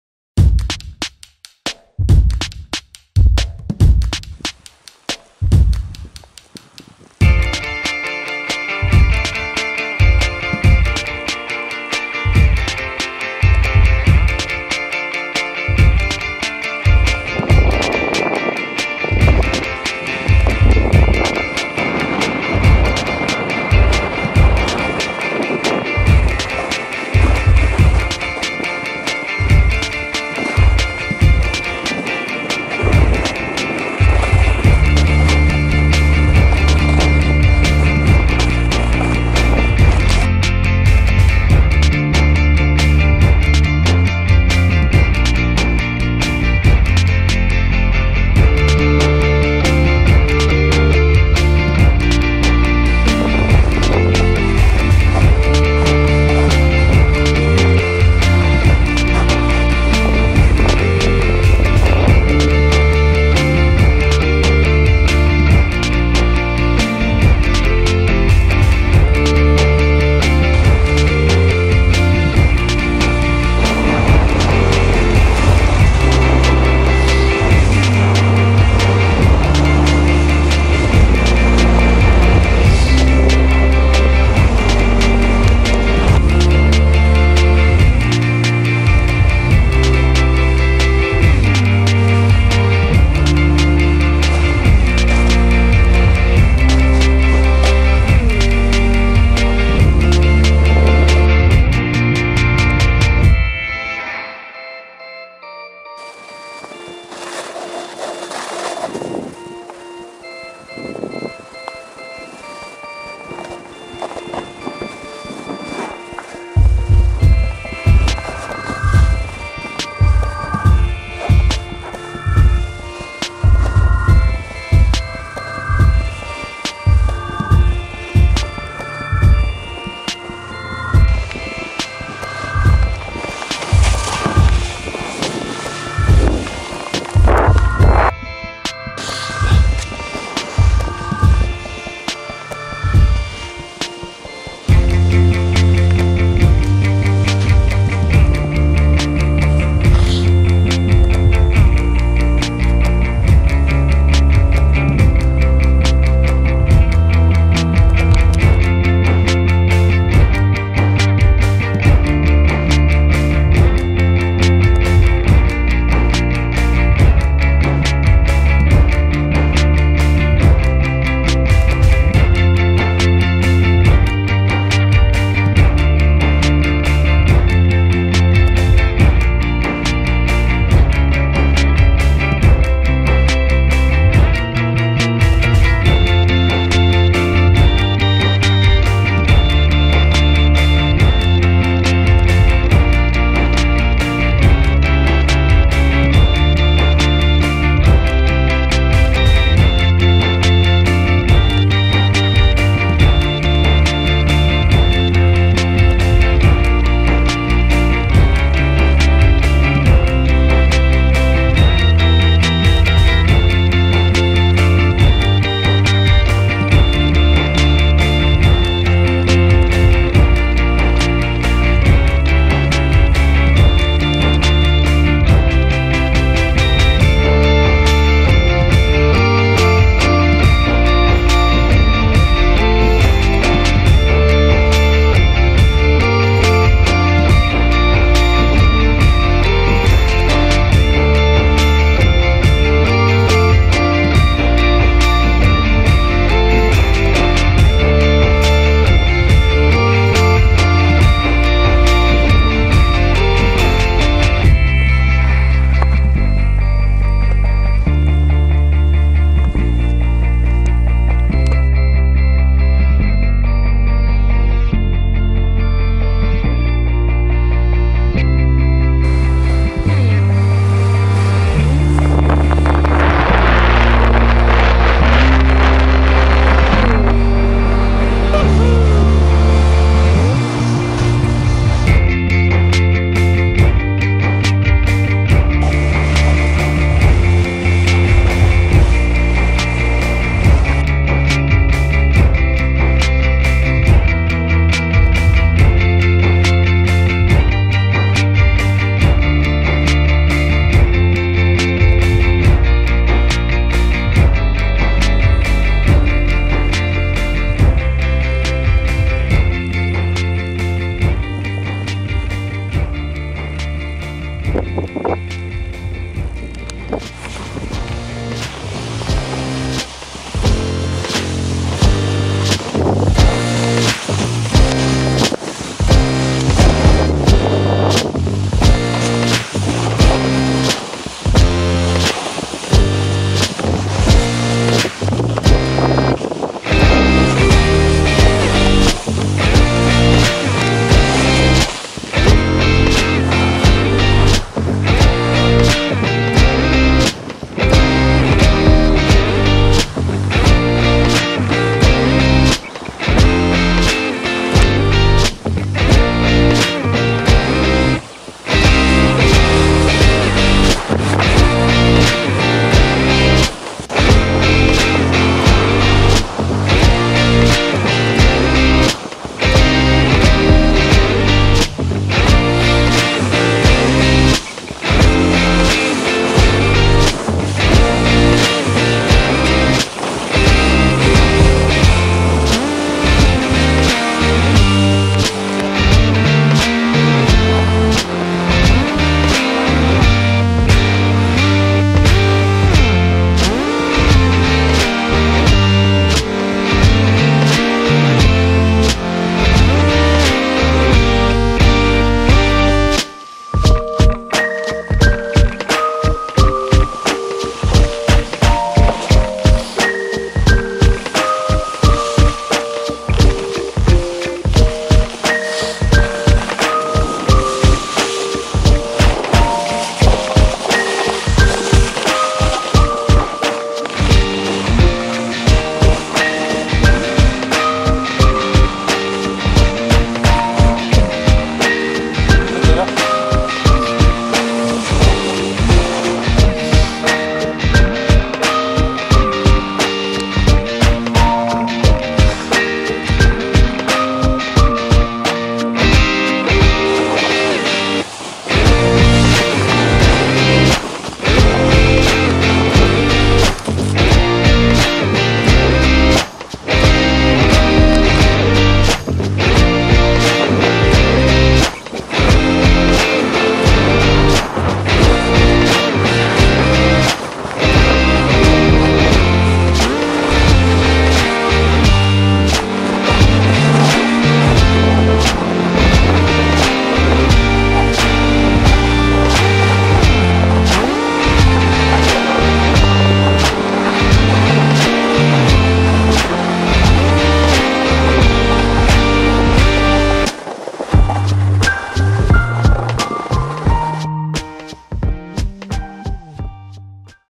COURCHEVEL BEST SNOWBOARD FREERIDE - COL DES SAULCES @ LA VANOISE
Rando jusqu'au col de Saulces, Rocher de Plassa, Petit Mont Blanc, Dents de Portetta, Combe des Roches, la Grande Casse, Chalet de la Grande Val, Refuge des Plans, Pralognan, les Avals etc...